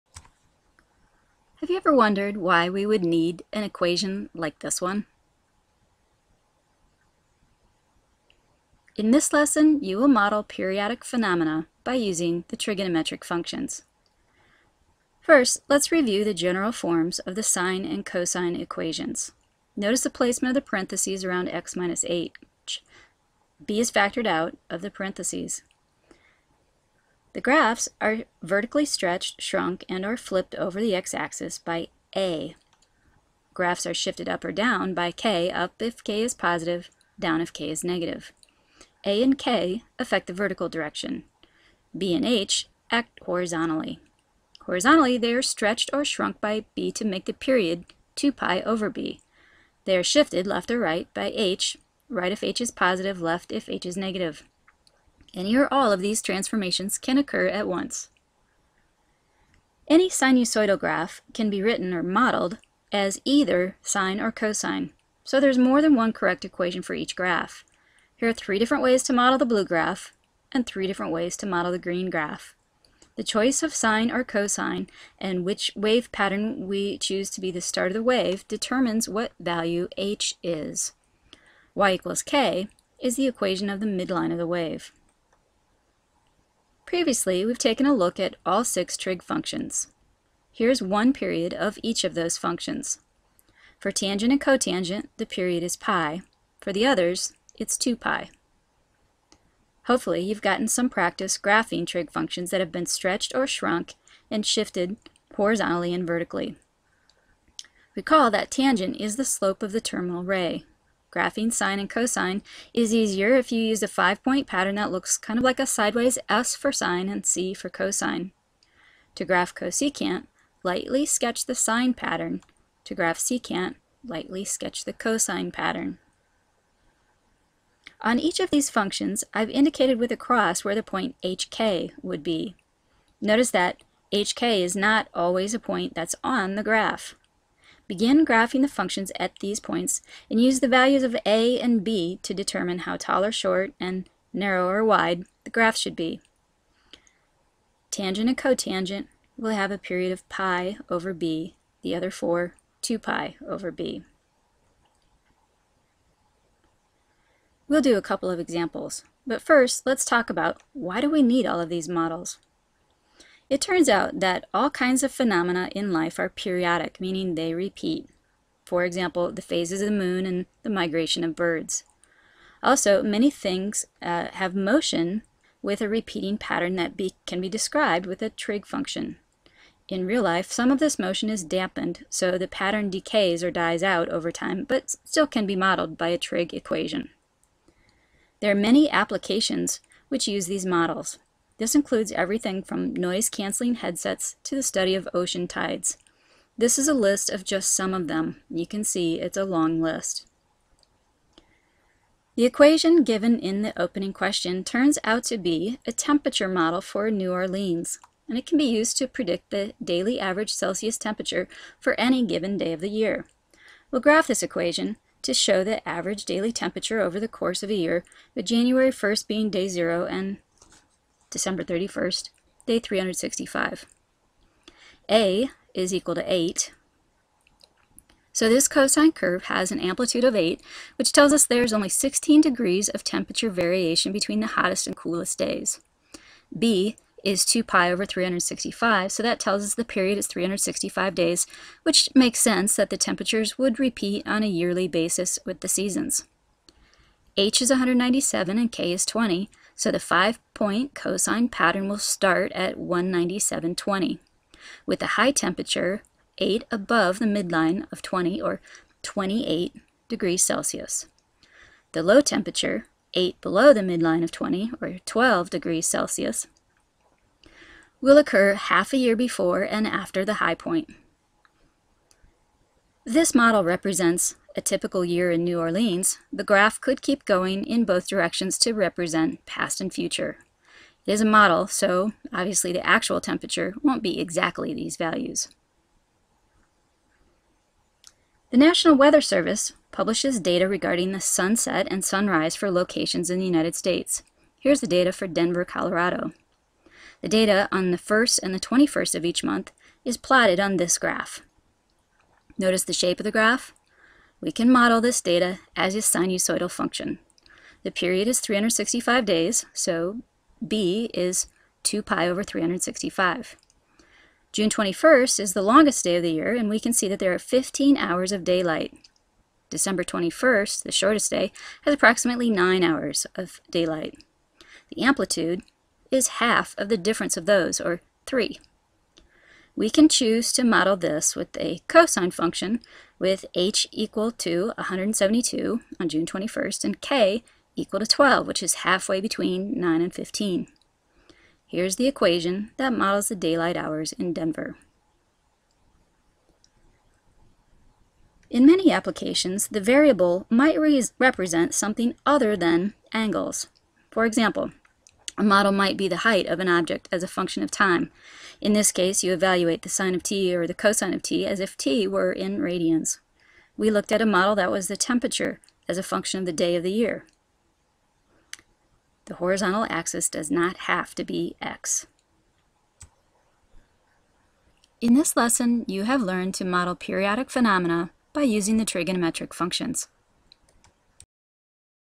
Model periodic phenomena using trigonometric functions
In this lesson you will learn to model periodic phenomena by using the trigonometric functions.

ADDITIONAL MATERIALS

STANDARDS

VA.GTF.T.3.c sketch the graph of the function by using transformations for at least a two-period interval; and

CCSS.HSF-TF.B.5 Choose trigonometric functions to model periodic phenomena with specified amplitude, frequency, and midline.

FL.MAFS.912.F-TF.2.5 Choose trigonometric functions to model periodic phenomena with specified amplitude, frequency, and midline.

IN.TR.PF.2 Graph trigonometric functions with and without technology. Use the graphs to model and analyze periodic phenomena, stating amplitude, period, frequency, phase shift, and midline (vertical shift).